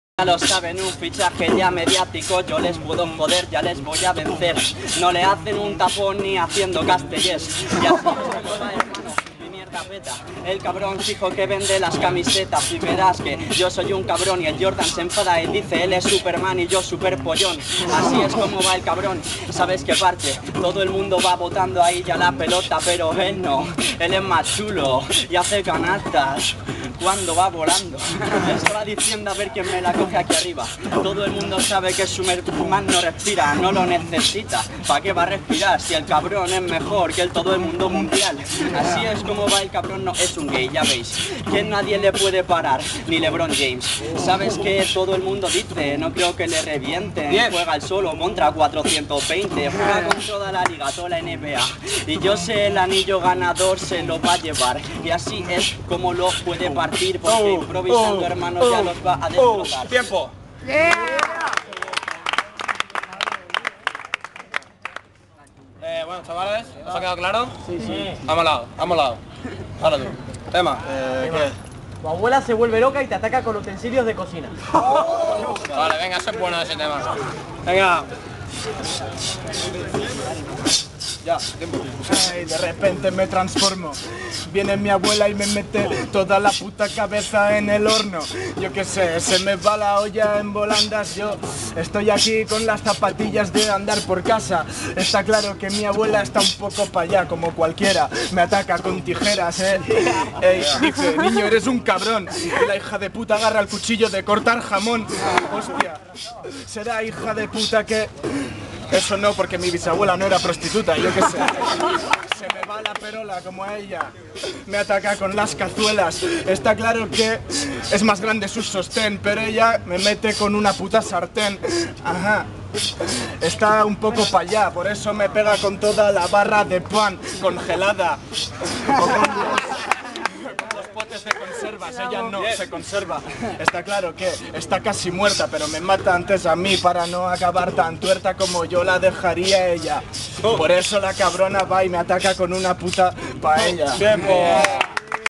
Woks vs Virux - Batallas por temas Macba 08/09/09
Gana Woks.

 - Tema Woks: Superman es fichado por los Lakers.

 - Tema Virux: Tu abuela se vueleve loca y te ataca con utensilios de cocina.